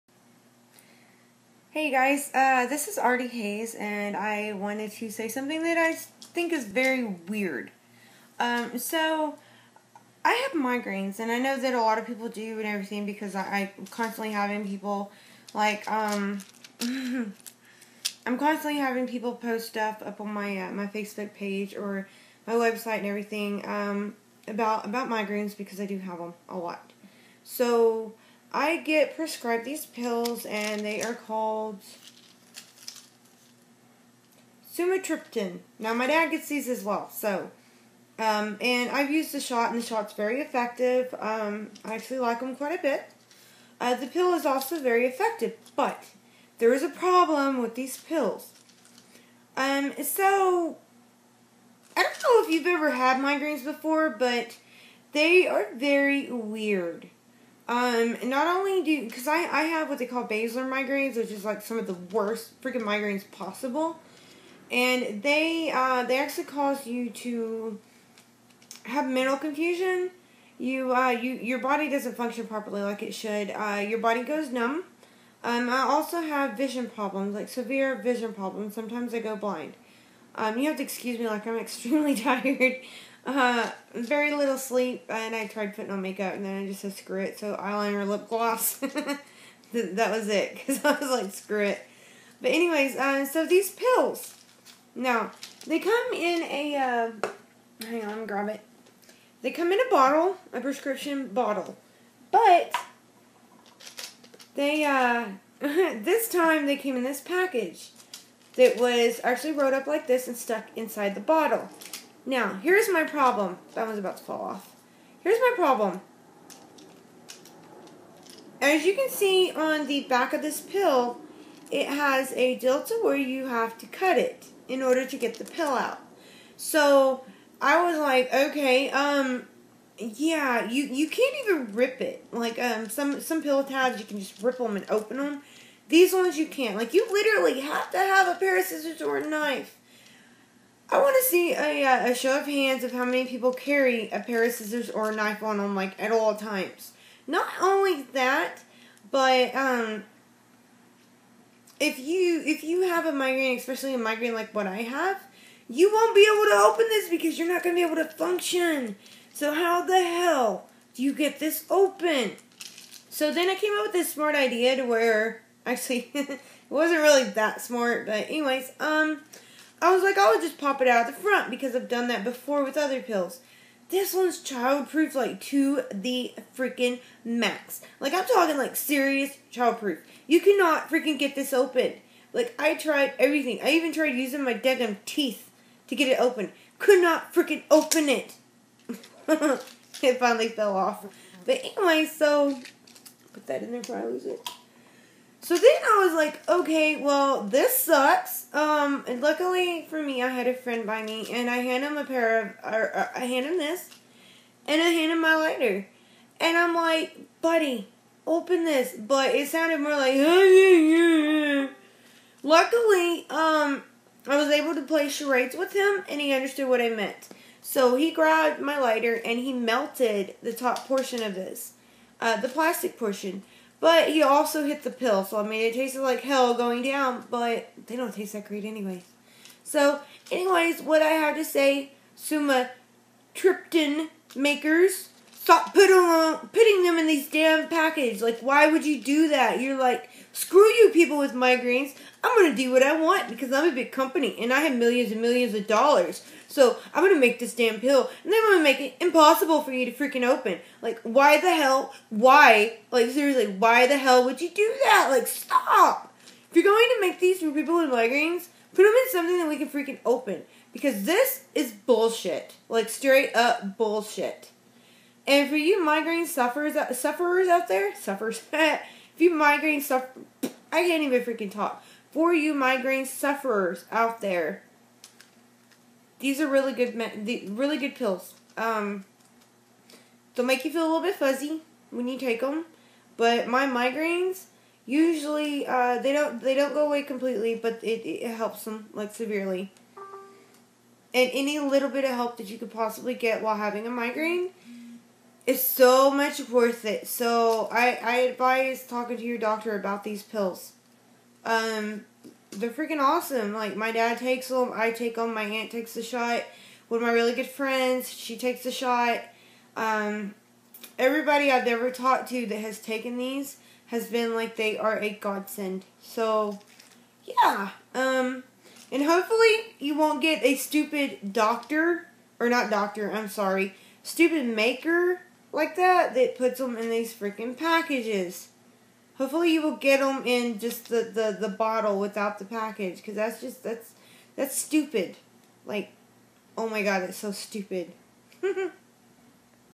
Migraine medicine's strange packaging
Why would sumatriptan make a package for people with pills for migraines and make it impossible to open.